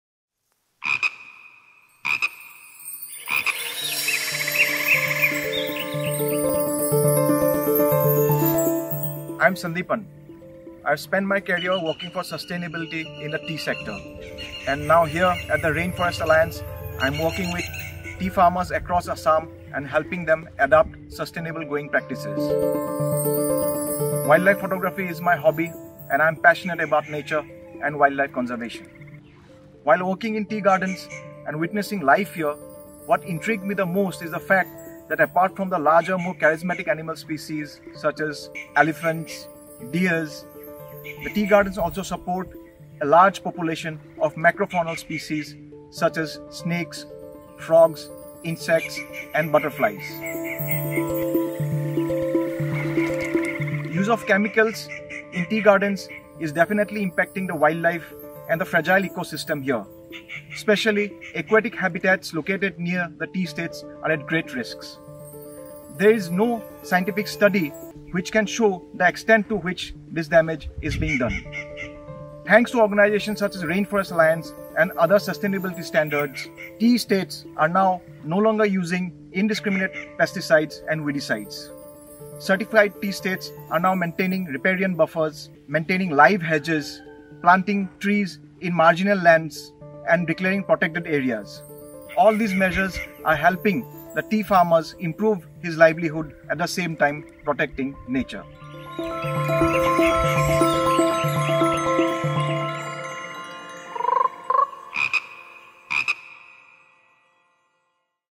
Celebrating the Wildlife of Assam’s Tea Estates
Sandeepan Bhattacharjee has spent his entire career working for sustainability in the tea sector. In his spare time, he has a hobby that takes him straight back to the tea estates: wildlife photography. Here’s his story.